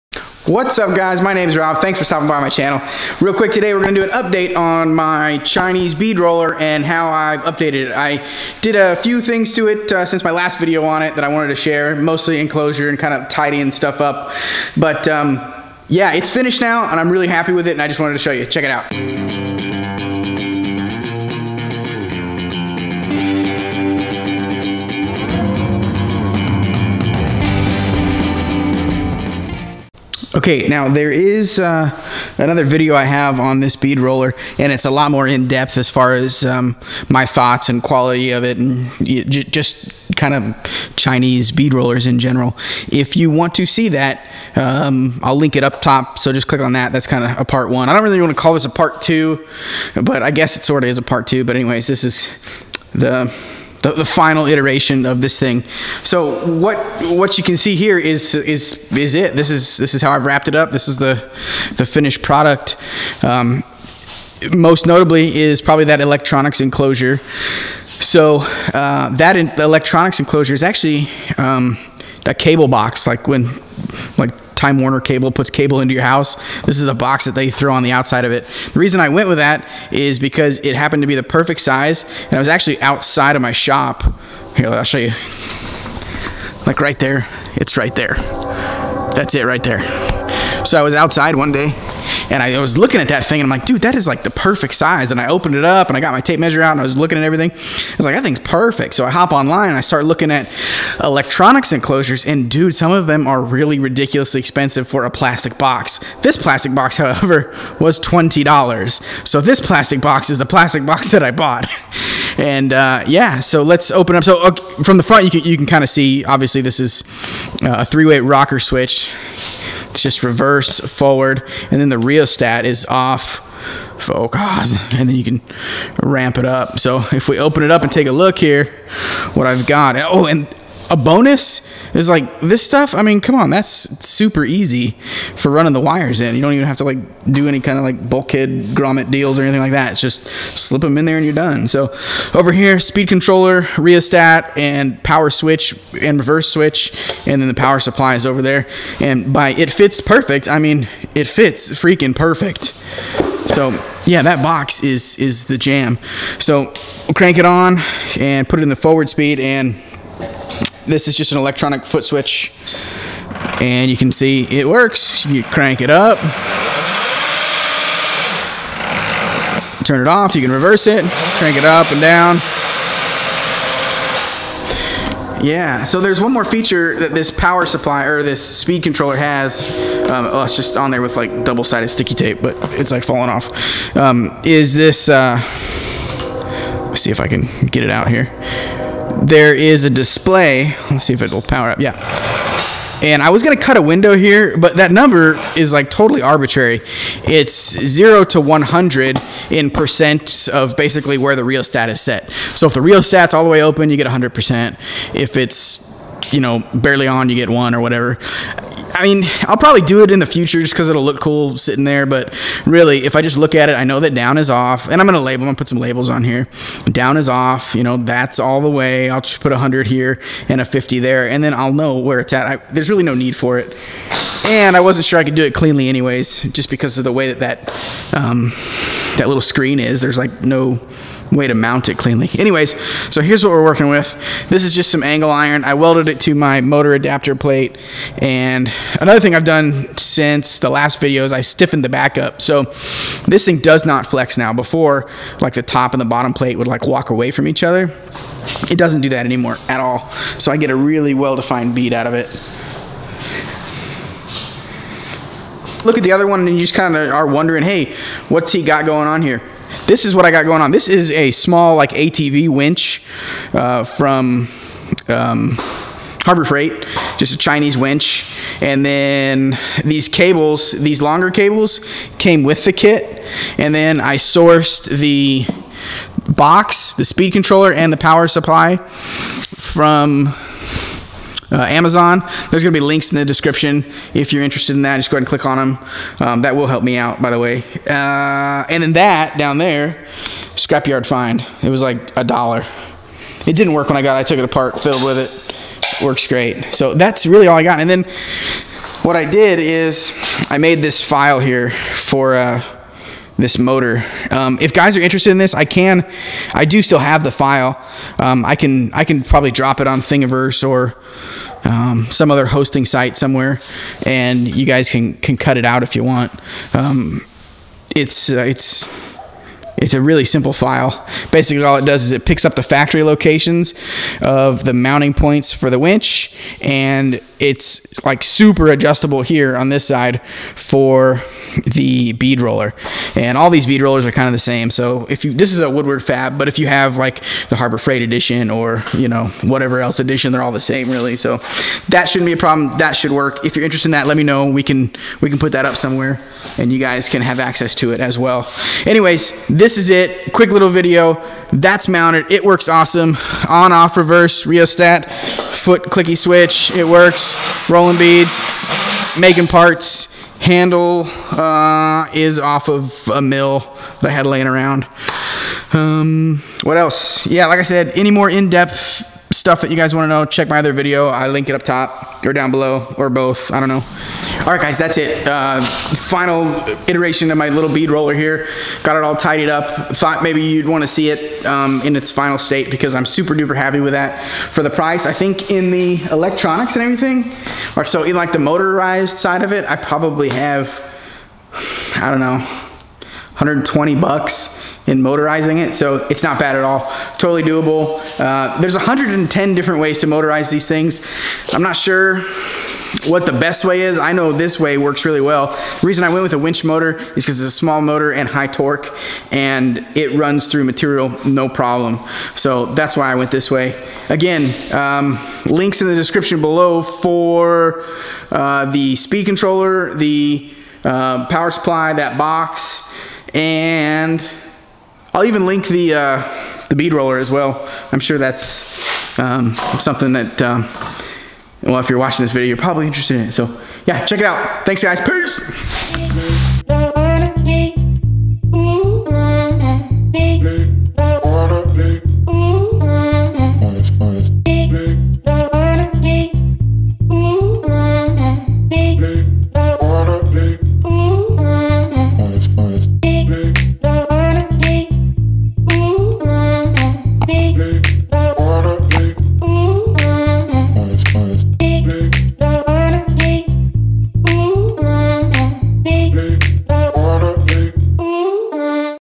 How to motorize a cheap Chinese bead roller
my cheap bead roller is in its final form!

Here are the parts I used to improve this tool.

Power supply
speed controller
Bead roller